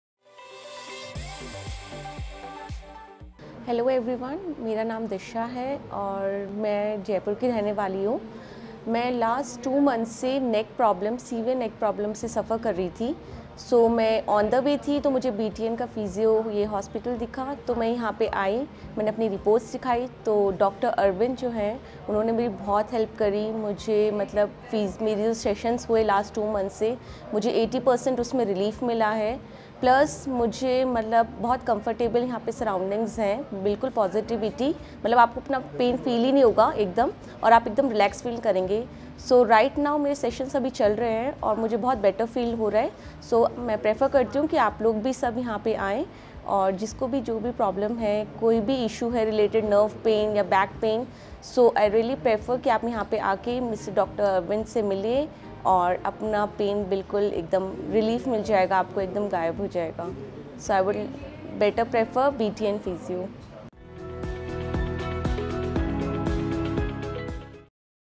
What Causes Neck Pain? Chronic Neck Pain Relief | Patient Testimonial | BTN Physio & Diagnostic Hub
Neck pain?
You don't need to live with chronic pain. We specialize in a wide variety of physical therapy treatments to get you back on track.

➡️Happy Patient Testimonial: Neck Pain Treatment.
➡️DM for Treatment & Consultation
➡️ Address -6/267, Sector 6 Malviya Nagar Rd, Jaipur Rajasthan 302017, India.